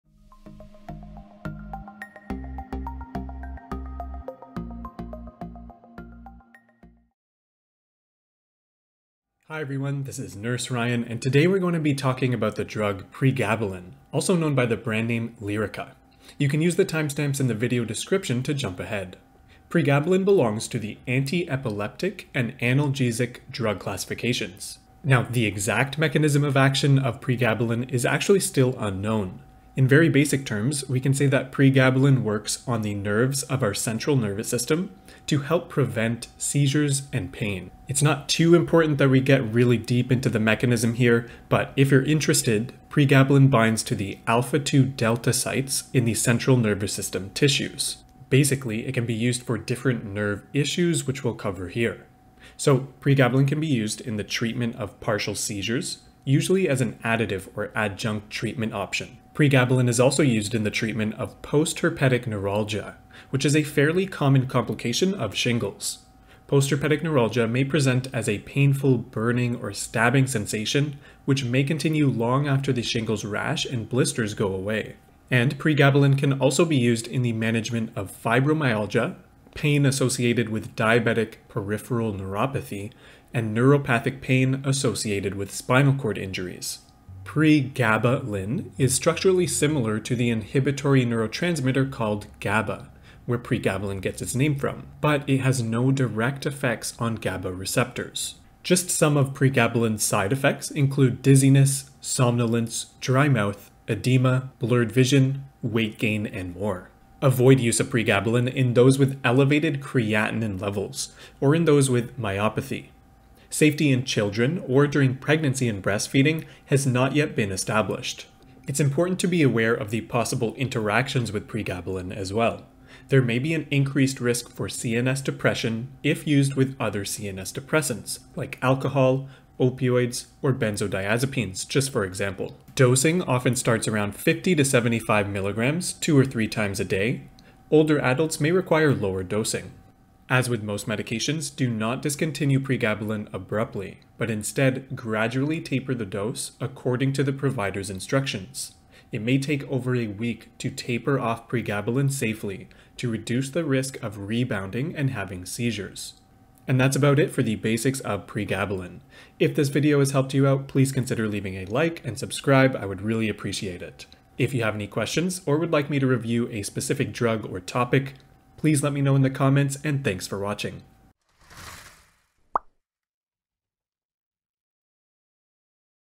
Pregabalin (Lyrica) Nursing Drug Card (Simplified) - Pharmacology
NCLEX Review: Pregabalin (Lyrica) - Mechanism of Action, Uses, Side effects, Contraindications, and Nursing considerations

Action: 0:19
Uses: 0:52
Side effects: 1:38
Contraindications: 1:47
Nursing considerations: 1:58

Reference Images:
Seizure (brain) image: epilepsyqueensland.com.au - What is epilepsy?

Shingles image: creakyjoints.org - Living with Arthritis

Intro: adMJ707
Outro: adMJ707